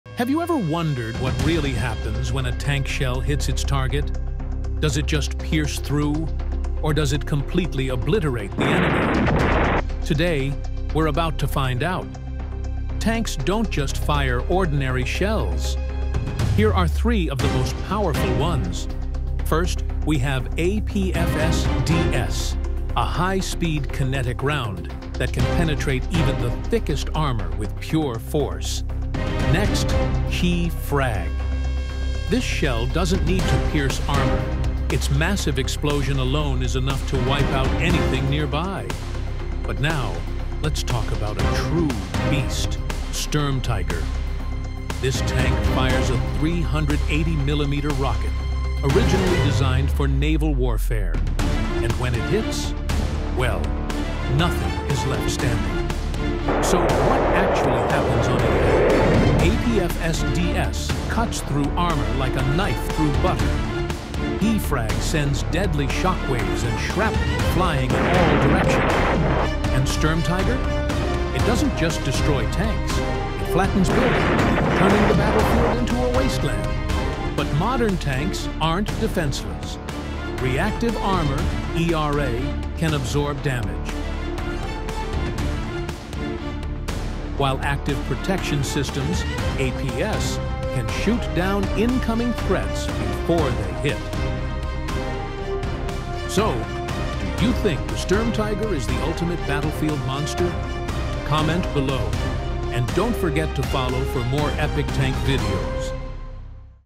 What Happens When a Tank Hits Its Target? The Truth Will Shock You
Do tank shells just pierce through, or do they completely obliterate their targets? 🤯
In this video, we break down three powerful rounds:
⚡ APFSDS – Kinetic energy that slices through armor!
🔥 HEAT – A molten jet that melts through steel!
💥 Sturmtiger – A 380mm rocket that flattens everything!
Can modern tanks survive with ERA and APS? Watch till the end and comment your thoughts! 💬
🔔 LIKE & FOLLOW for more epic tank content! 🚀